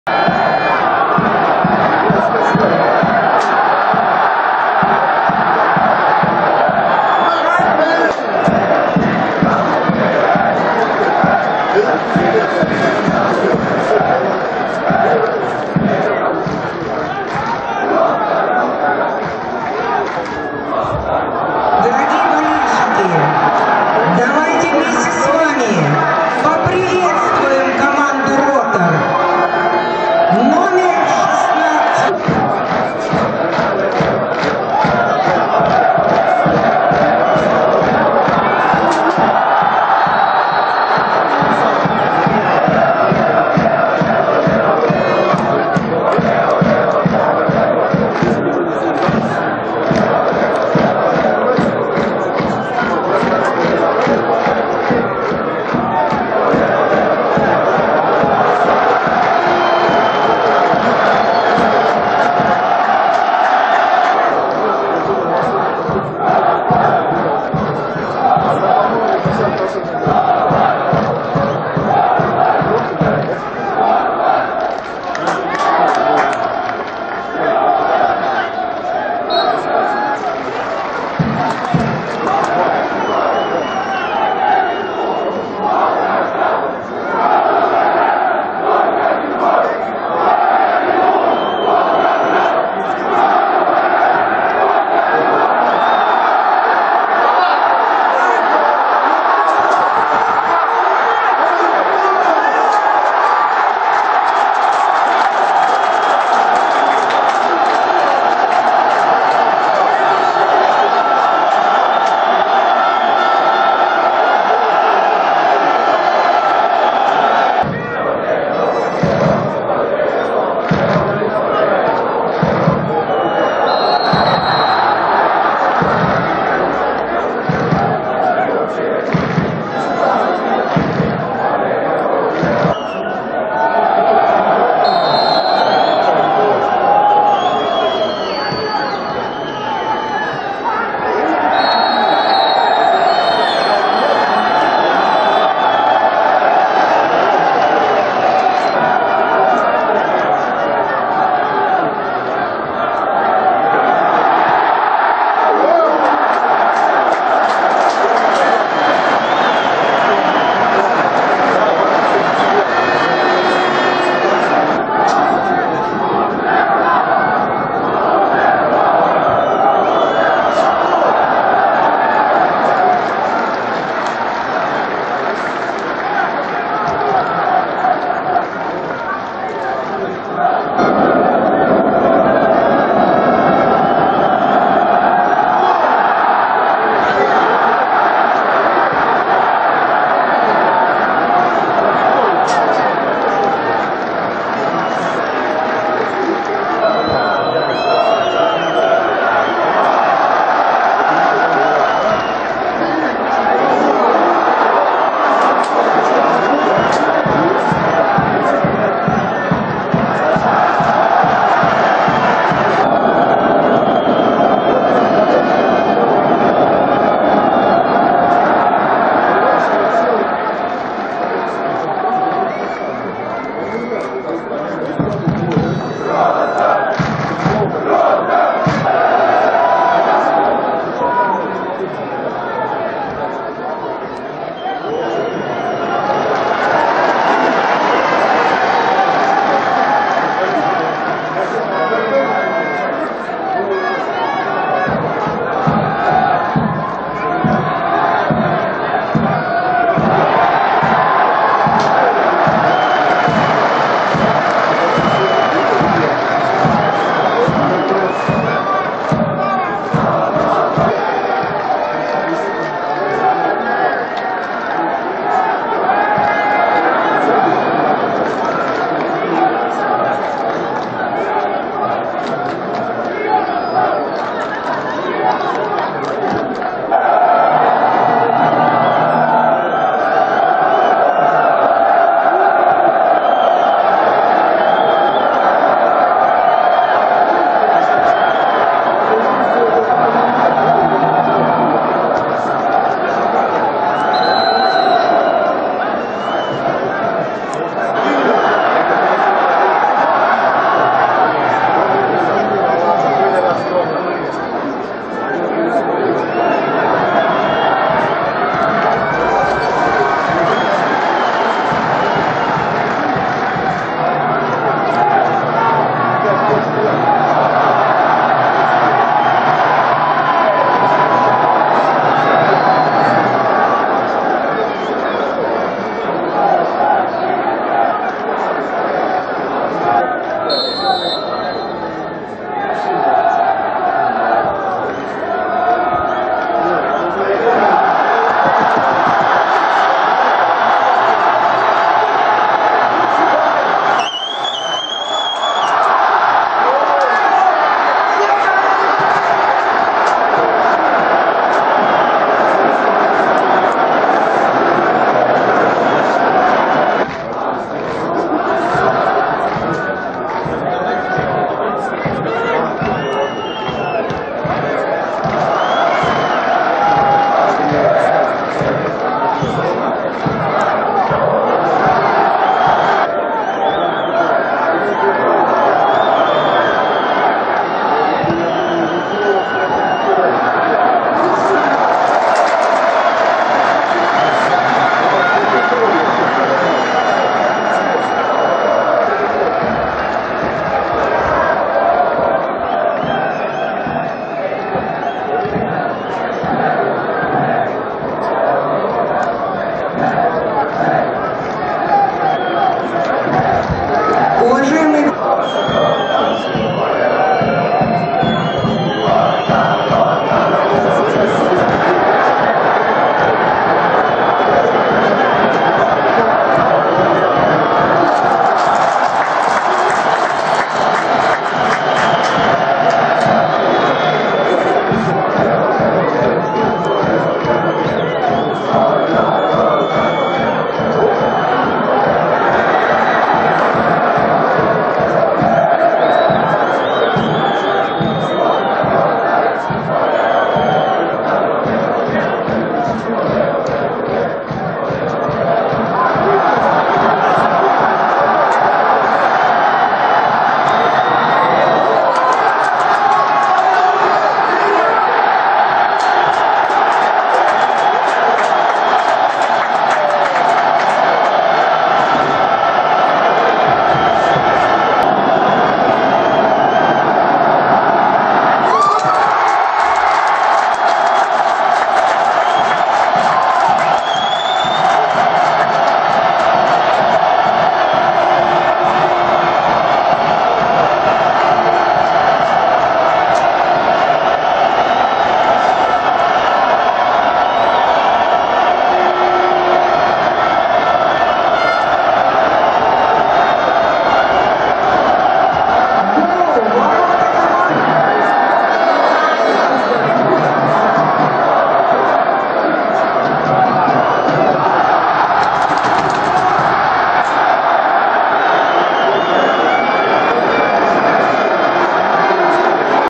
Ротор - Шинник (Первенство ФНЛ 2013/2014)
Дата проведения матча: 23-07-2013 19:30
Голы: Аппаев, 19 (1:0). Архипов, 90+ - с пенальти (2:0).
Волгоград. Стадион «Центральный». 9000 зрителей.